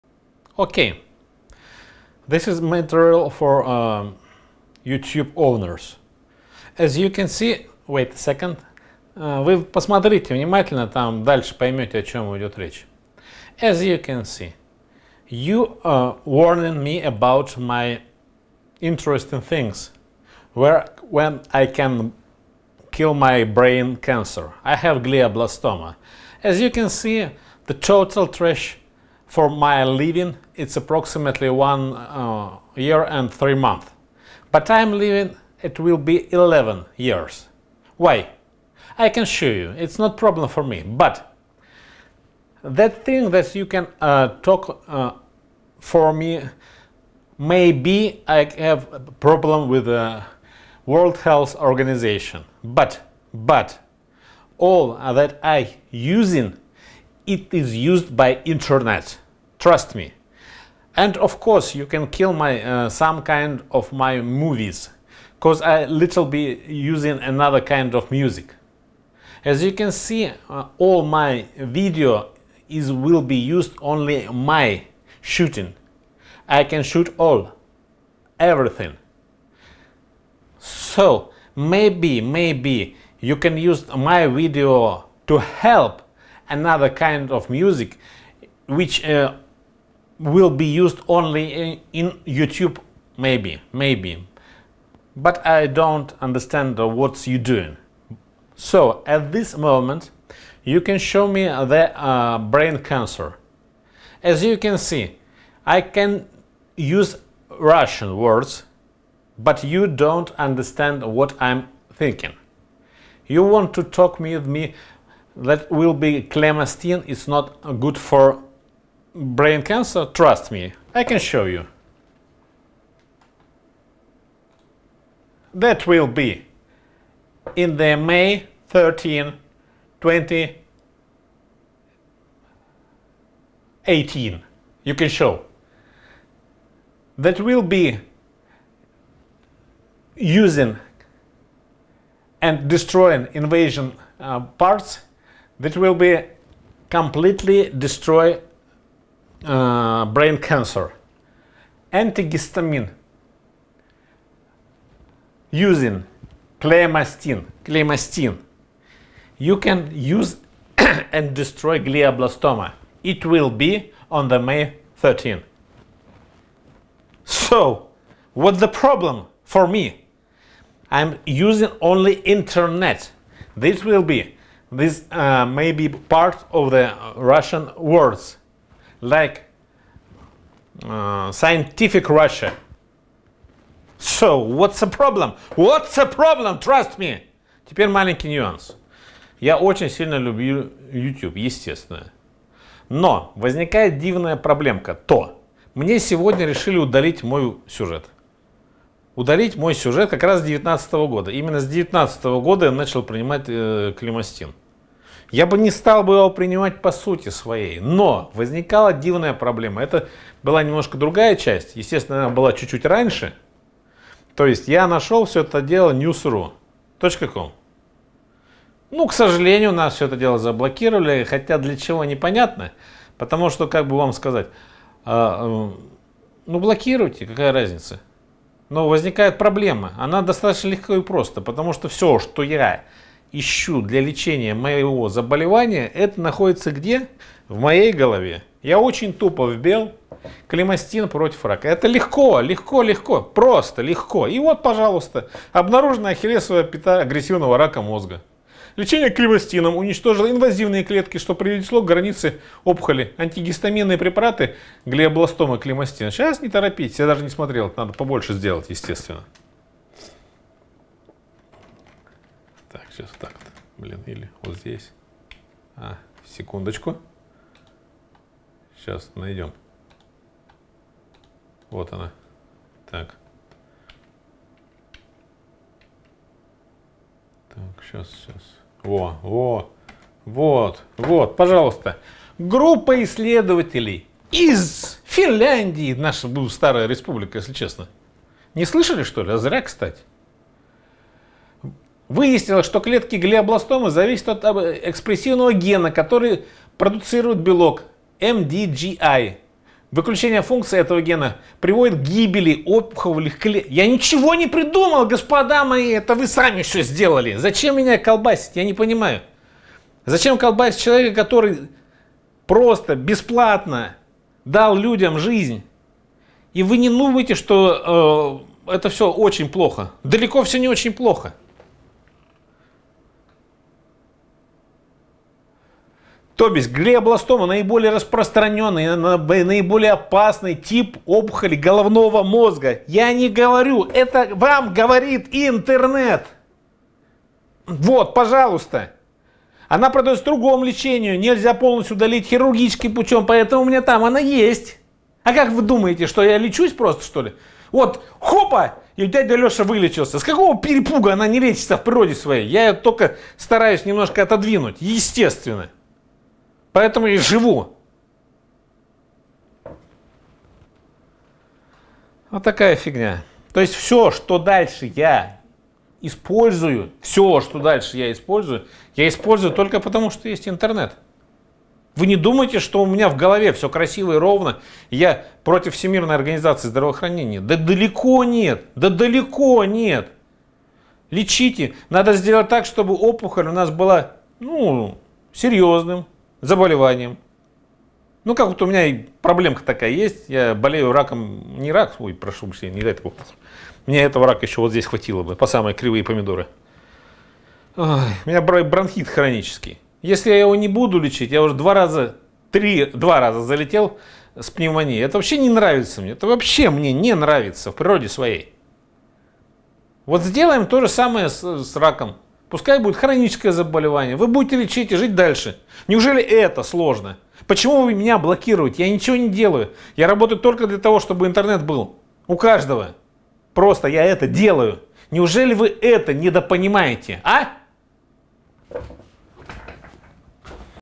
Glioblastoma using internet
Челейкин Алексей - интернет рассказывает как лечить Глиобластому в 2019м году, Cheleykin Alexey - the thretment for Glioblastoma using internet in 2019 years